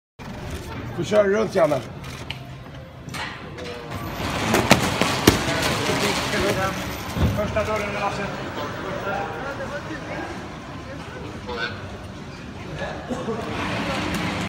Europa League Final - Zlatan Ibrahimovic Arrives In Style!
Zlatan Ibrahimovic is transported through the Friends Arena, Stockholm, on a buggie ahead of Manchester United's Europa League final against Ajax.

Thanks